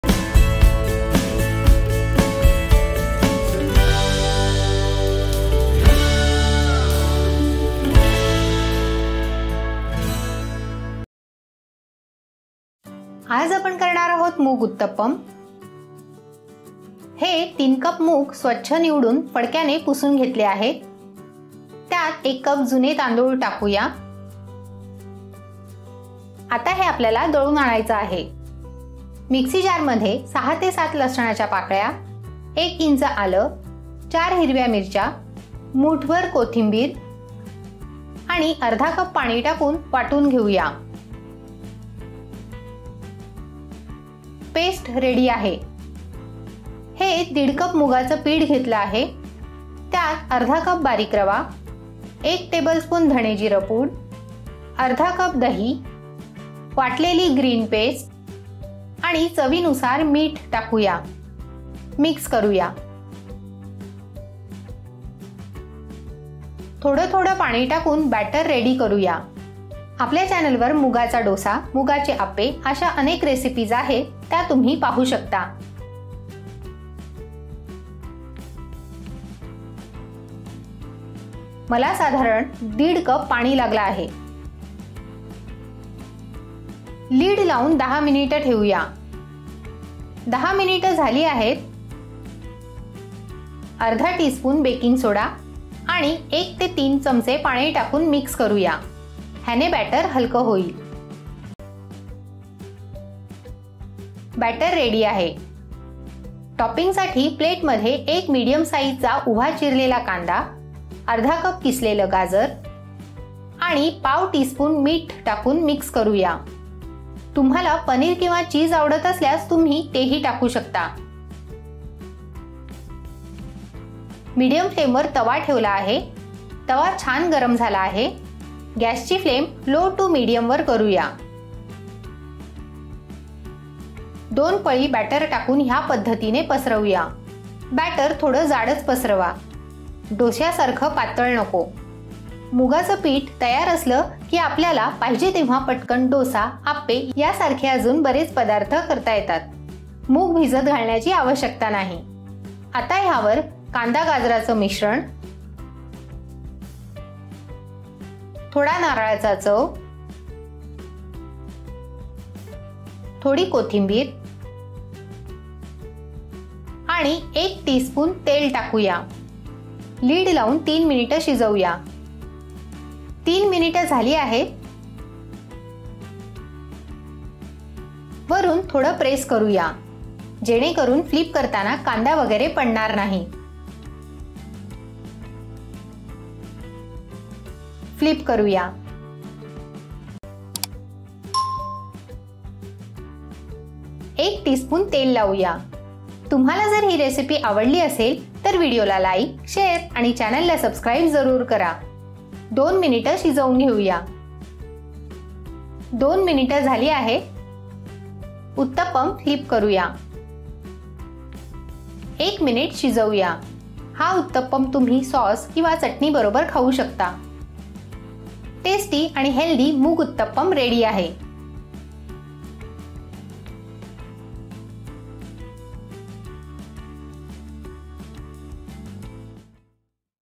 Mugacha Uttapam Recipe |Moong uttapam in Marathi |Instant Uttapam recipe| Mug Uttapam| मुगाचे उत्तपम
Mug Uttapam or Moong Uttapam is a popular South Indian recipe. It is a soft and fluffy pancake recipe that is very nutritious and keeps you full for a long time. It is super easy and quick to prepare as it requires very few ingredients. You can serve these savoury pancakes with chutney to make a wonderful breakfast or light meal. In this video, I will show you how to make this delicious recipe at home. I hope you try this out and let me know how it turns out.